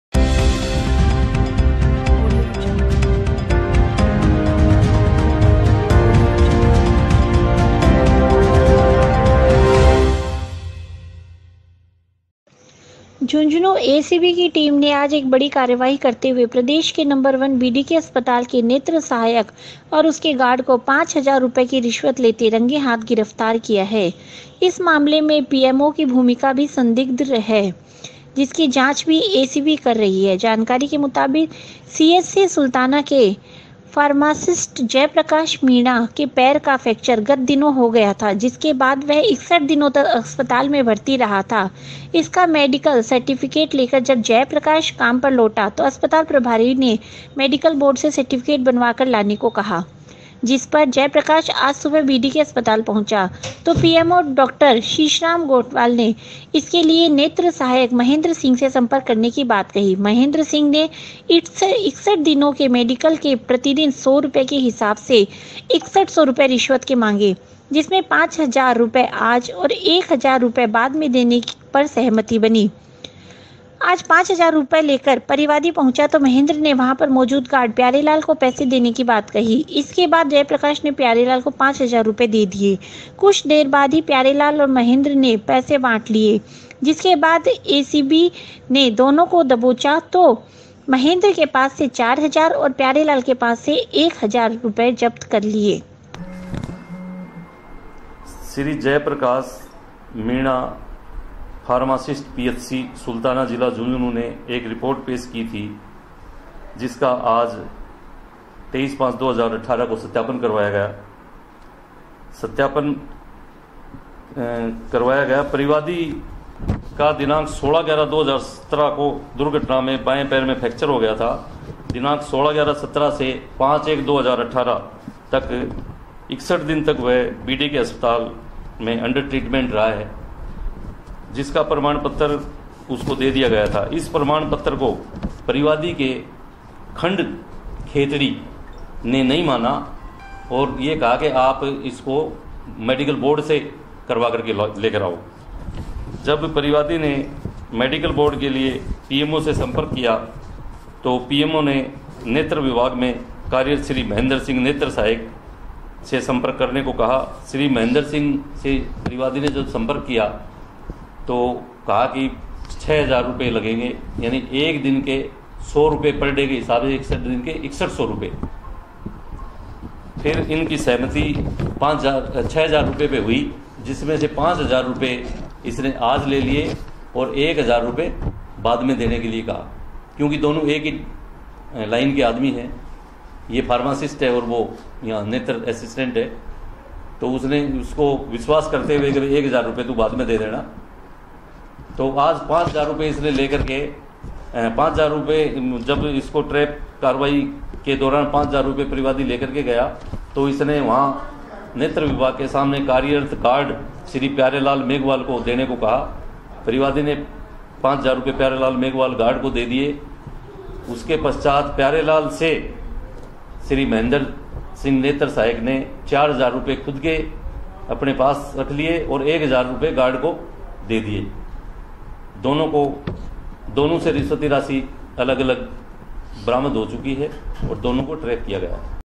JHUNJHUNU I एसीबी की कार्रवाई
झुंझुनूं एसीबी की टीम ने आज एक बड़ी कार्रवाई करते हुए प्रदेष के नंबर वन बीडीके अस्पताल के नेत्र सहायक और उसके गार्ड को पांच हजार रूपए की रिष्वत लेते रंगे हाथ गिरफ्तार किया है। इस मामले में पीएमओ की भूमिका भी संदिग्ध है। जिसकी जांच भी एसीबी कर रही है।